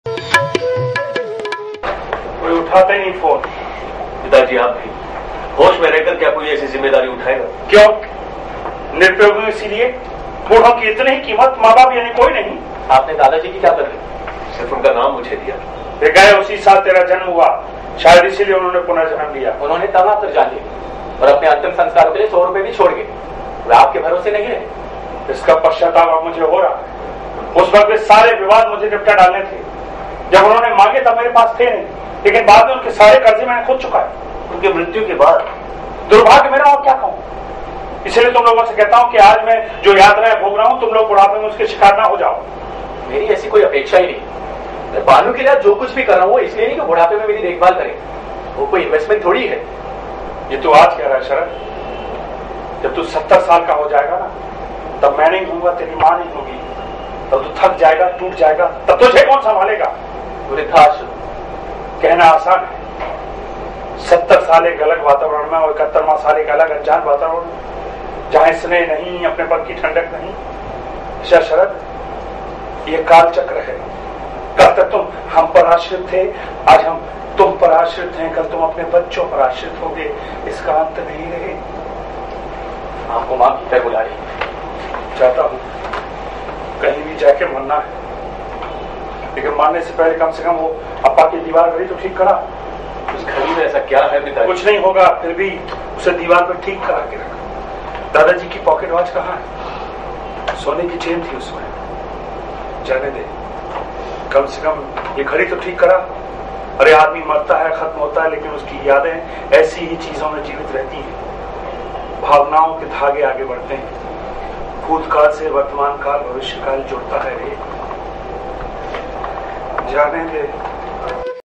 Kaal Chakra - Hindi Play
Video of Kaal Chakra - Hindi Play

Cast :

Om Katare, Paromita Chatterjee, Raaj Shetty, Deepshikha, Shreyas Shah, Rishi Khurana, Damini Kanwal, Ashok Sharma.

Writer : Jayant Dalvi

Design & Direction : Om Katare

Synopsis :
A play focusing on the mental and physical anguish that old couples undergo due to sheer callousness on the part of their children who do not wish to be burdened with the responsibility of looking after their aged-parents.